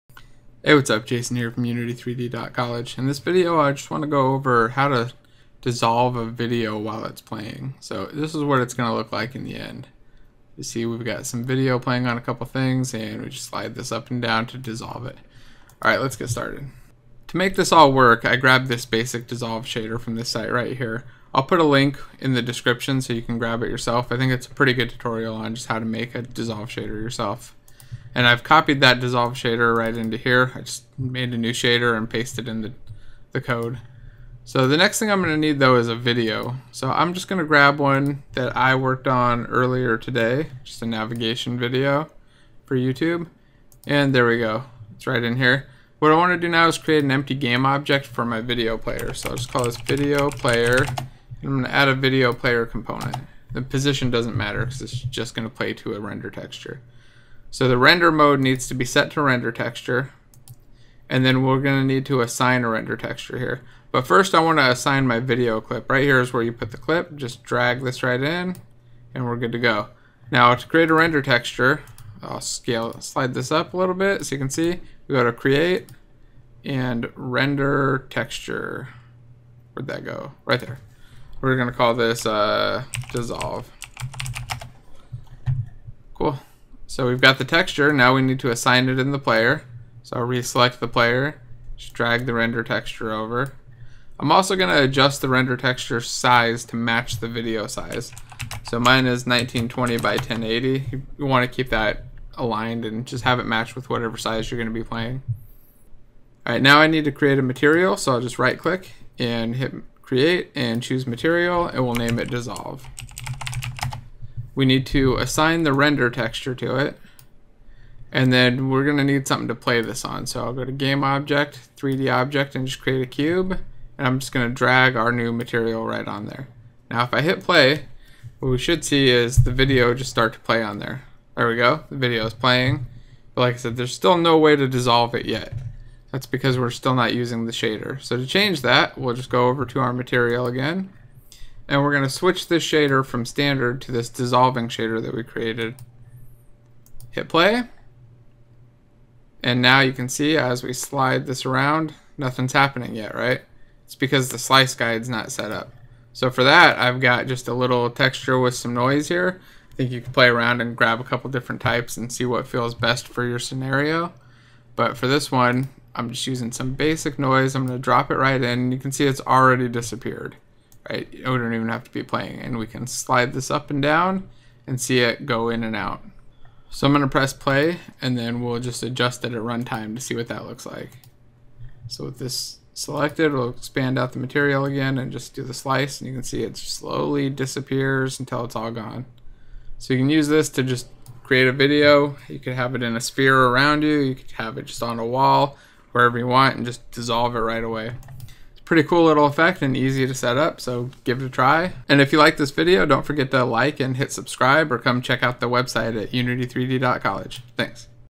Unity HowTo - Disolving Video with the VideoPlayer
Learn how to dissolve your video while playing with this simple shader & setup.